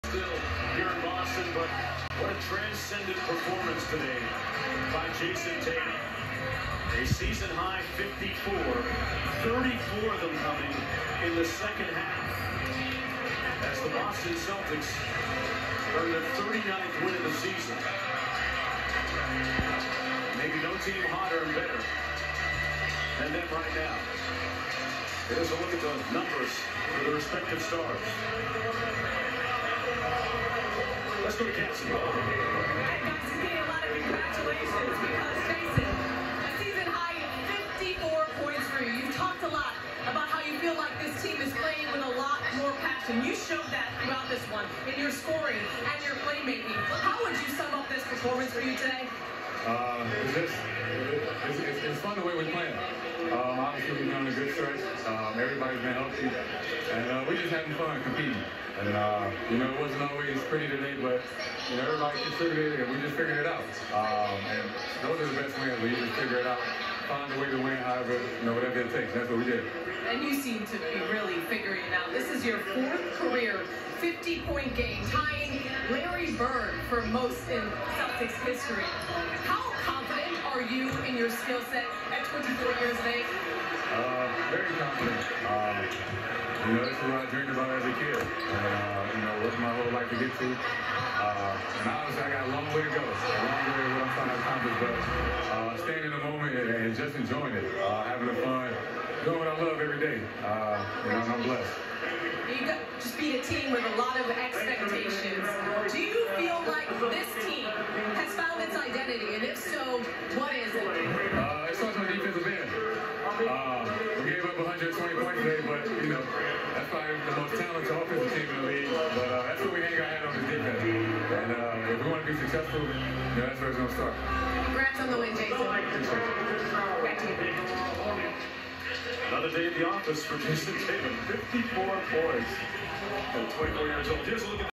Jayson Tatum season high with 54 points against Brooklyn Nets.
Ties Larry Bird for most 50 point games in Celtics history. Fourth career 50 point or more games.